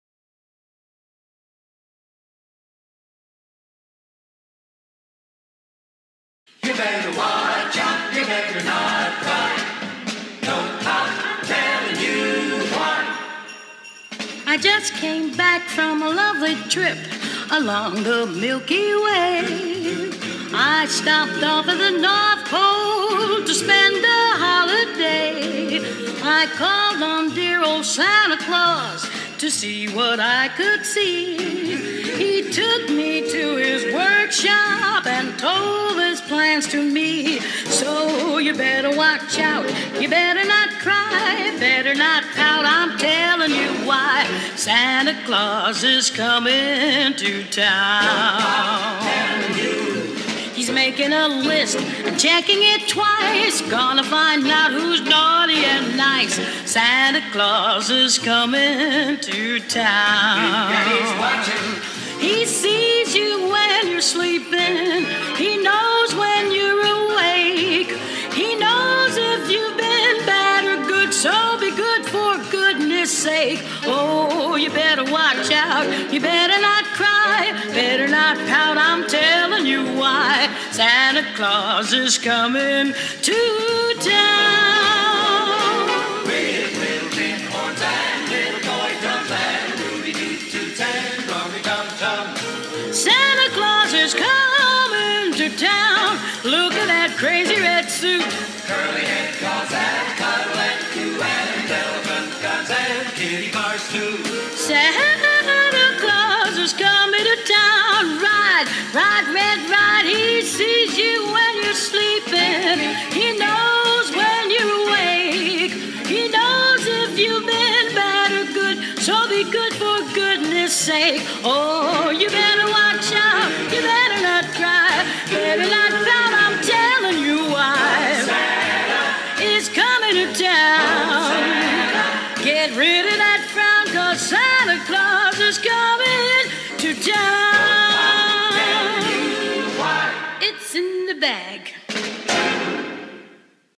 Parkside Primary Mannequin Challenge
The staff members at Parkside took a little break after school to have some holiday fun!  We learned a lot from our students!